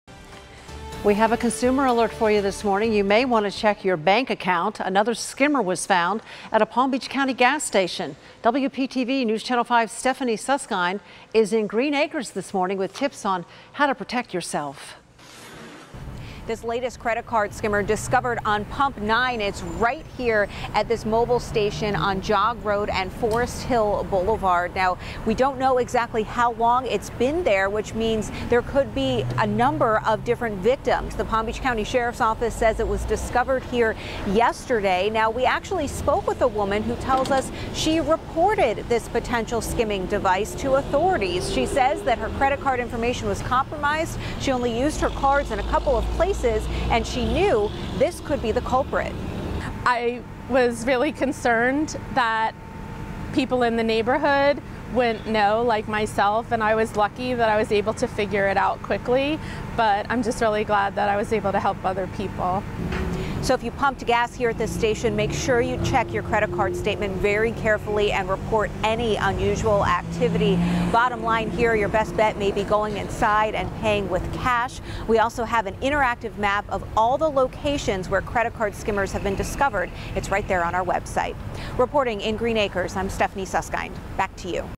Woman says she was victim of skimmer at Greenacres gas station
A Palm Beach County woman says she alerted authorities about a skimming device found at a Mobil gas station in Greenacres.